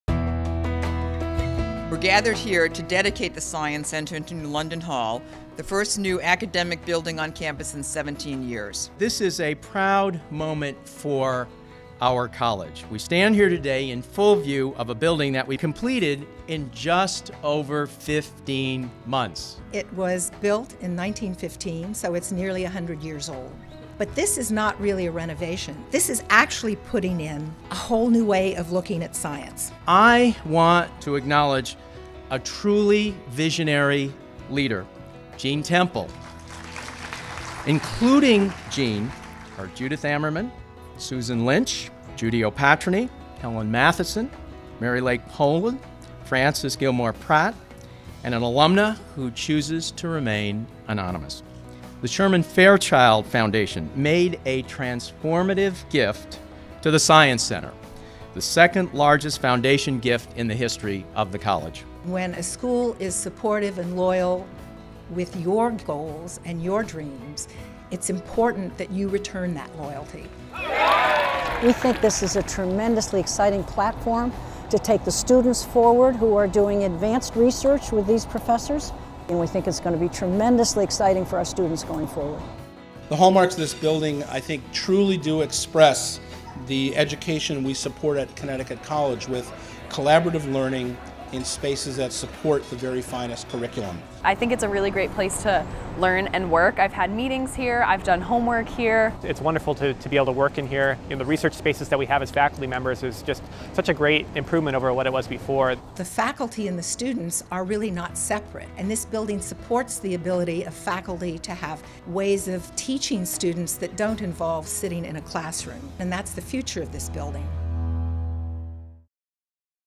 Connecticut College Science Center Dedication!
New London Hall, the College's first academic building, was transformed in 2011 and 2012 into a state of the art center for the life sciences and computer science.